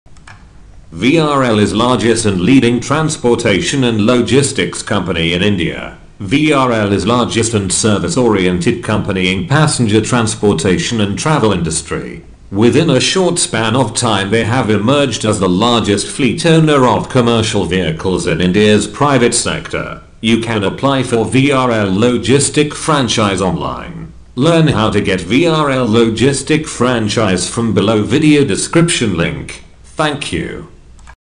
How To Open VRL Logistics Franchise - VRL Courier Agency
VRL is largest and leading transportation and logistics company in India, VRL is largest and service oriented company in passenger transportation and travel industry,Within a short span of time they have emerged as the largest fleet owner of commercial vehicles in India's private sector,You can apply for VRL logistic franchise online.